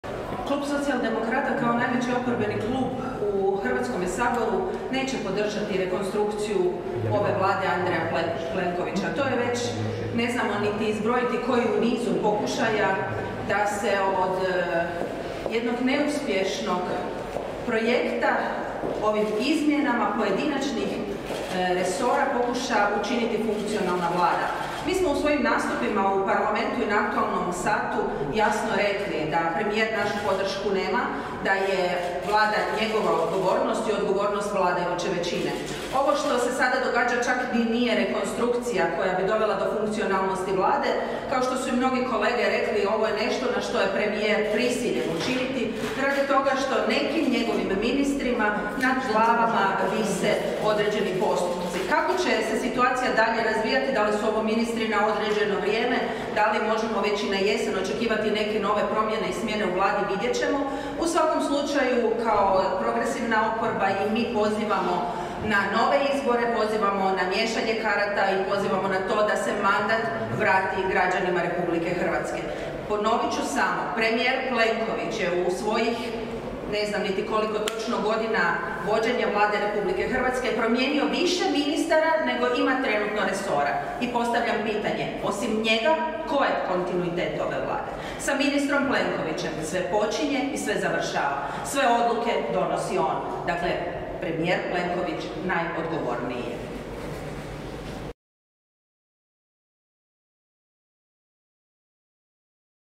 Ivana Posavec Krivec nakon saslušanja ministara: Nećemo podržati ovu rekonstrukciju Vlade
Na konferenciji za medije održanoj u Saboru zastupnica Ivana Posavac Krivec komentirala je saslušanje novih ministara.